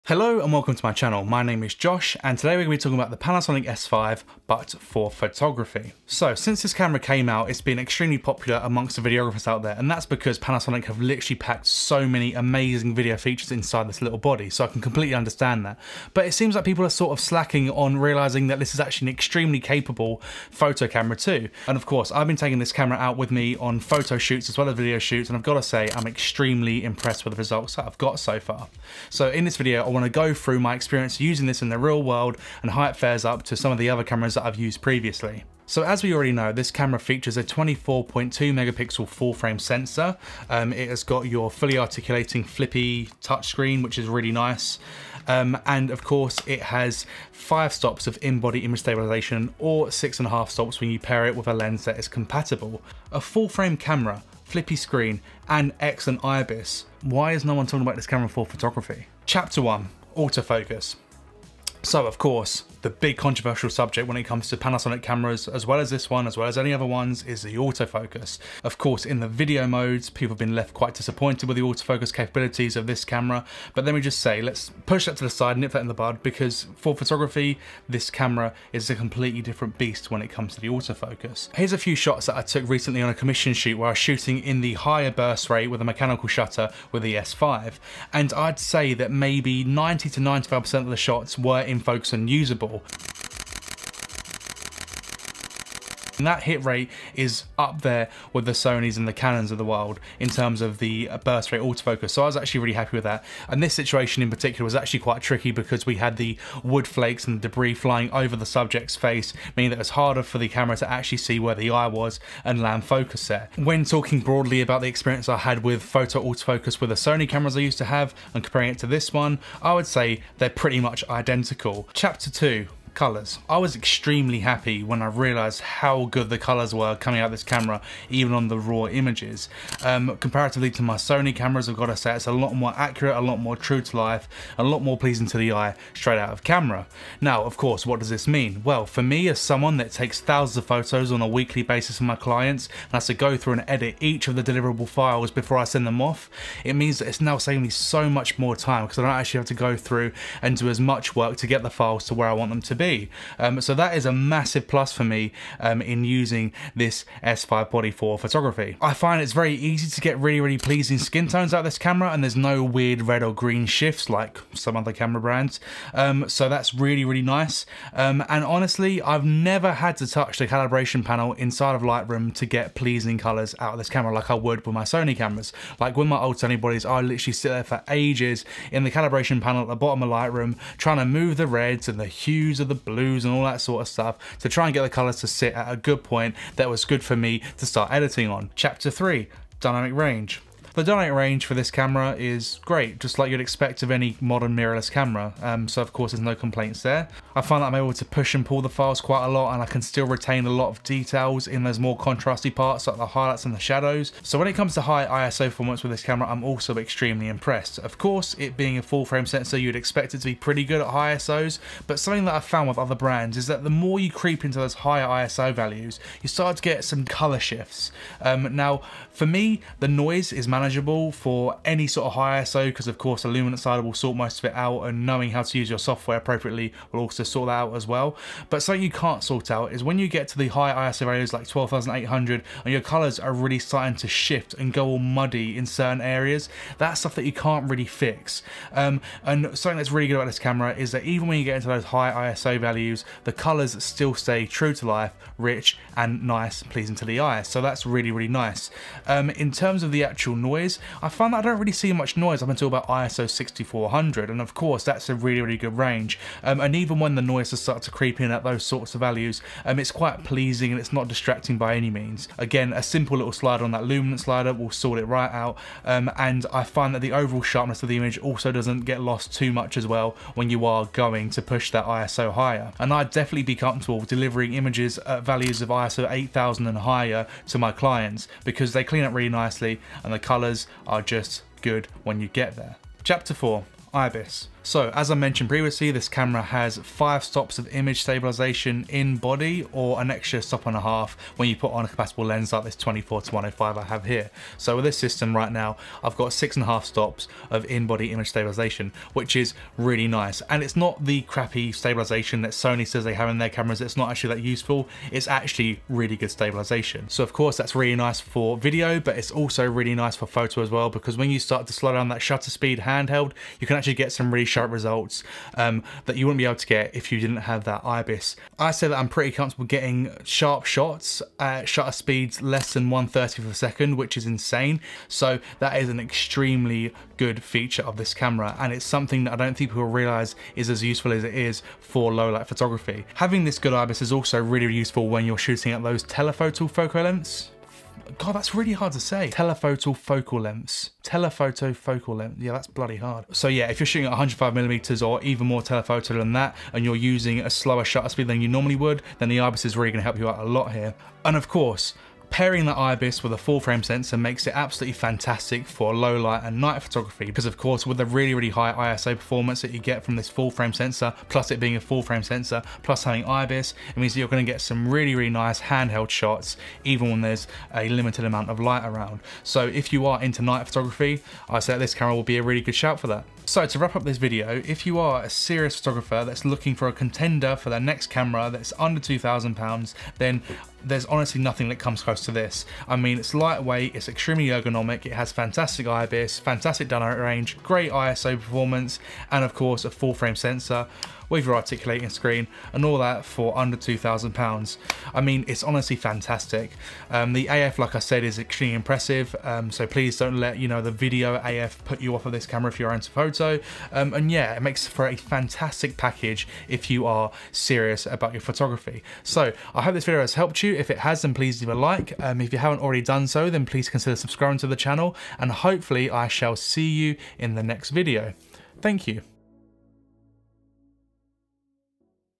Panasonic S5 - Not Just Great For Video... The Photographer's Perspective
Panasonic S 24-105 F4 OIS Macro Lens

No one seems to give this camera the credit it deserves for its photo capabilities, so I wanted to make a video to address that.

I shoot a mixture of photo and video content for my clients every week and the Panasonic S5 is the camera I use for everything. It's lightweight, comfortable to use and produces some of the nicest images I've been able to provide my clients with so far.

If you're a photographer and have around £2000 to spend on your next camera then definitely consider the Panasonic S5! It's a beast for the price point and I don't think there's much that comes close for the performance you get from it.

I really hope you find this video helpful if you are looking to buy a Panasonic S5 for photography, and please be sure to let me know if you'd like to see anything else about the Panasonic S5 on this channel.